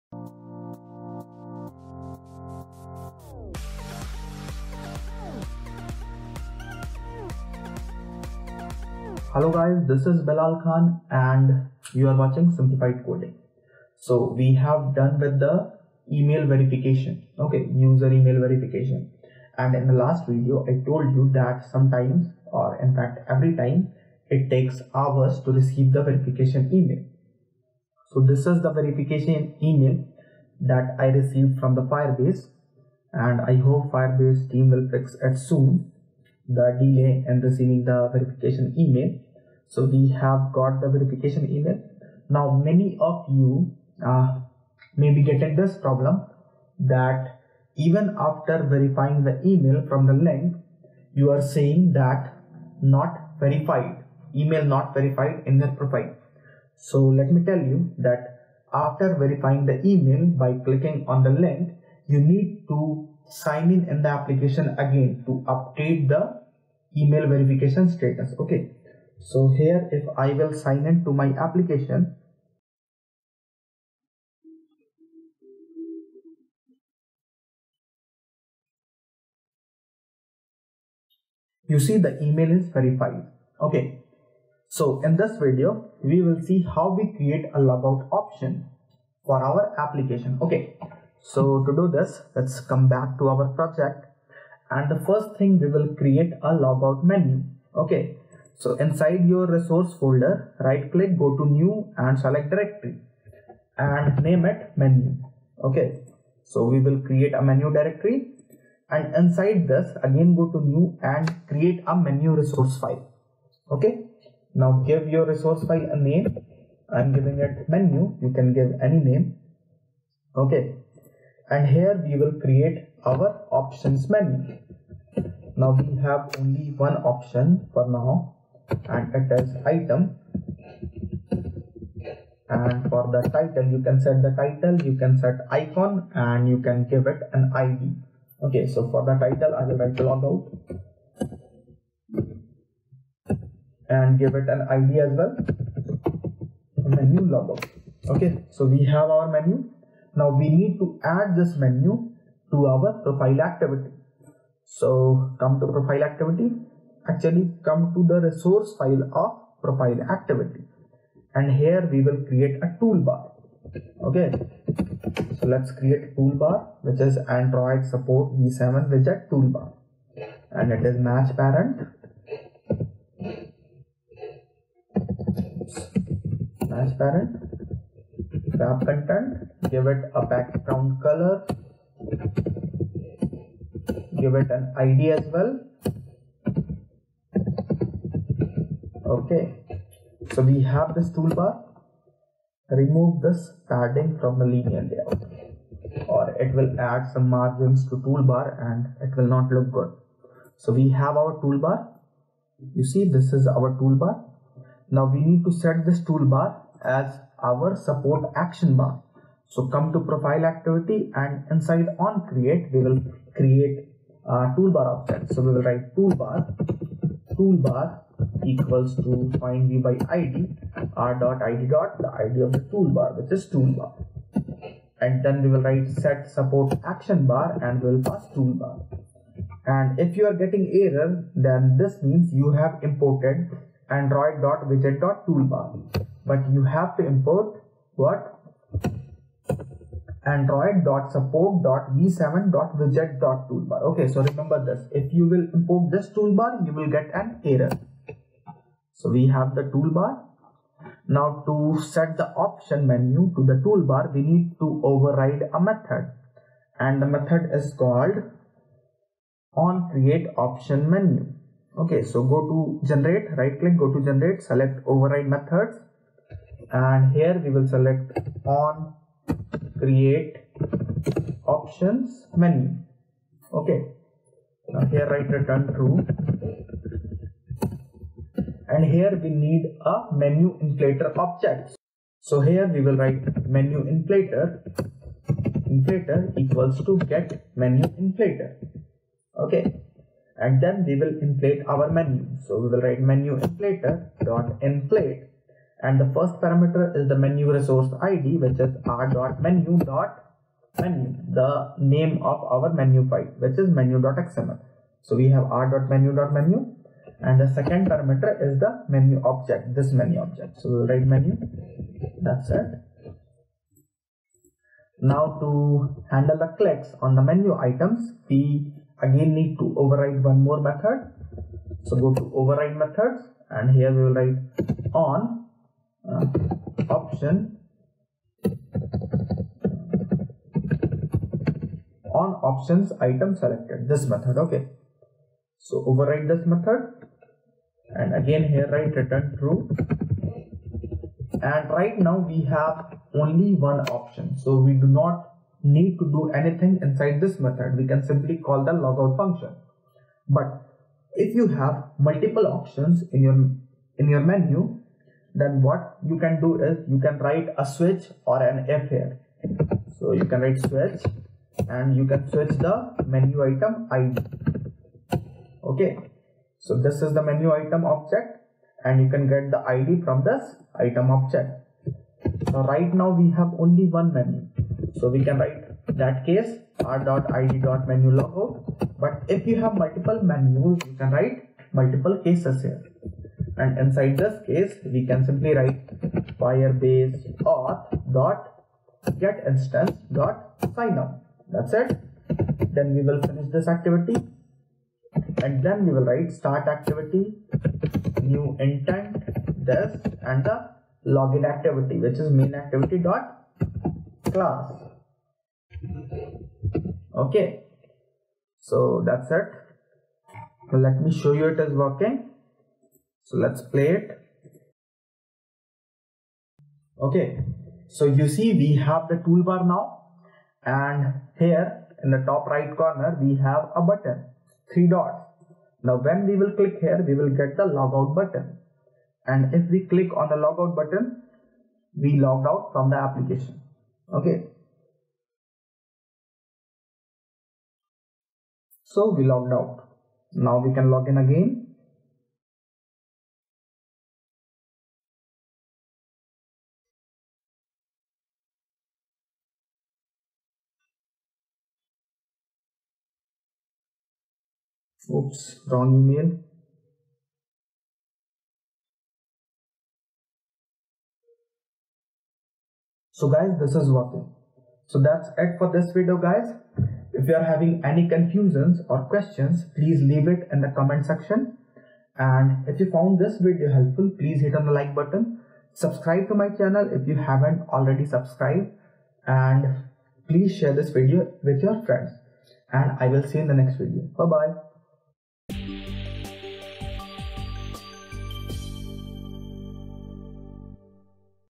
#6 Firebase Authentication Tutorial - SignOut Option
Firebase Authentication Tutorial [ Updated ]

Learn everything about Firebase Authentication. In this video, we will see how we can add a Sign Out option to the application.